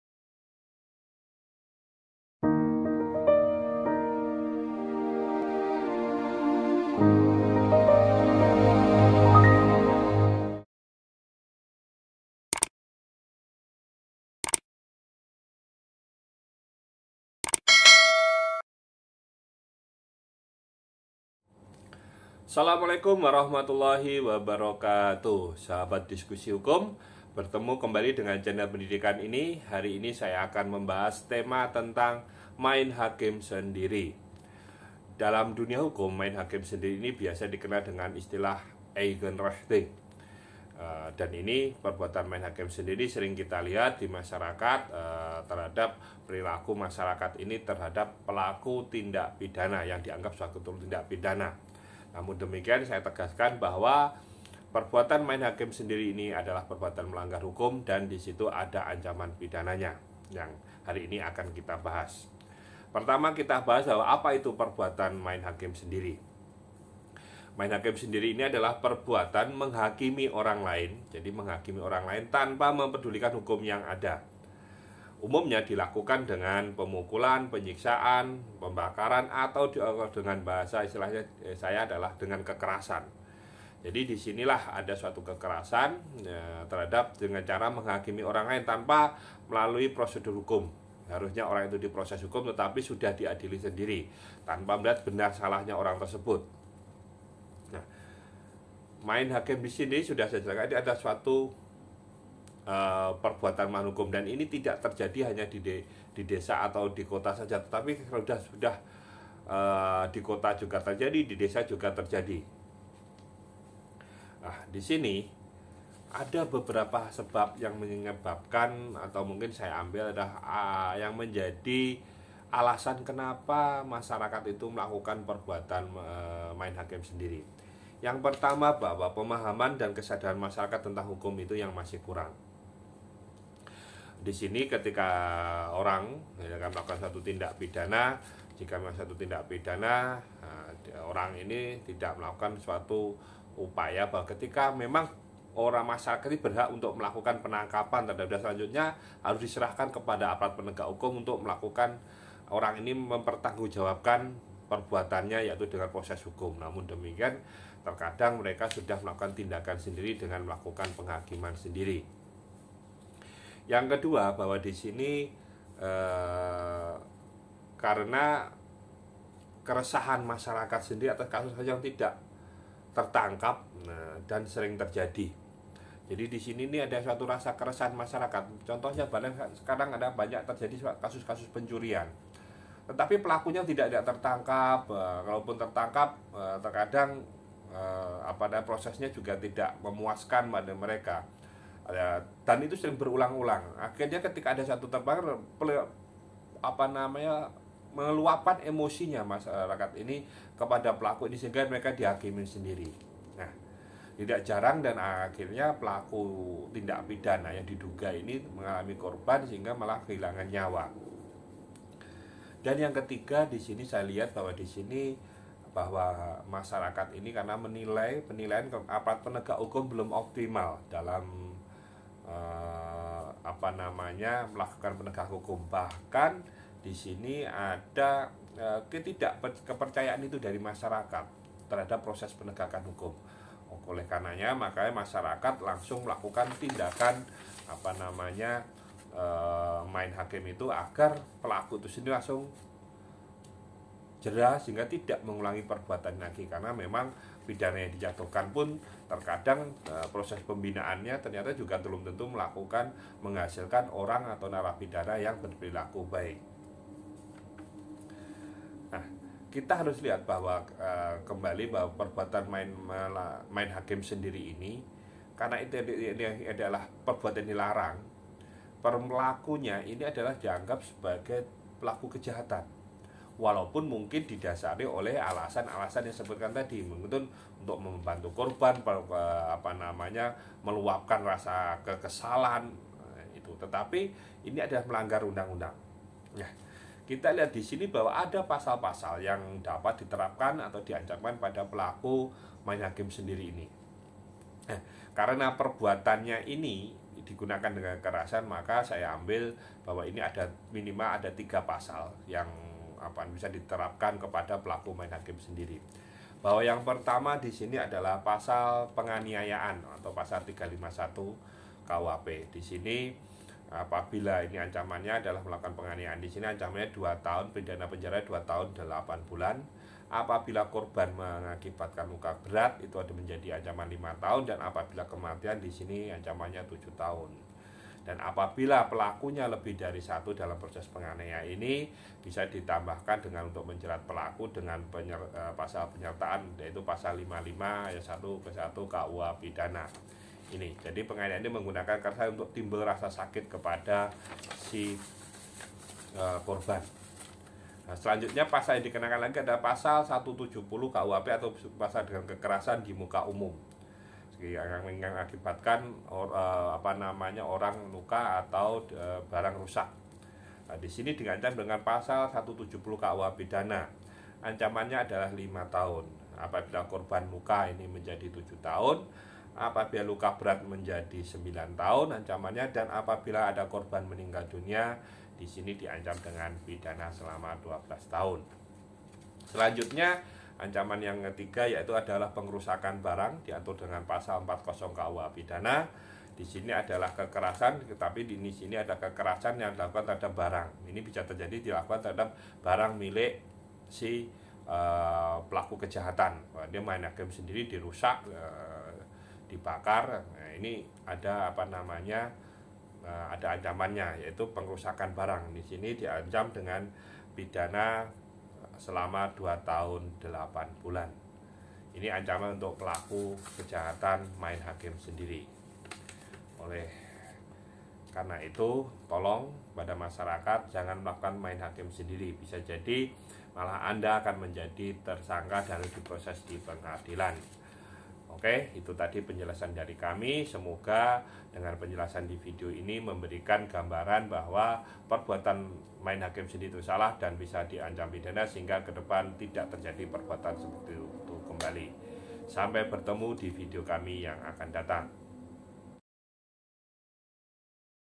HUKUMAN PELAKU MAIN HAKIM SENDIRI
main hakim sendiri adalah tidak dibenarkan dan merupakan perbuatan melawan hukum. ada ancaman pidana buat pelaku main hakim sendiri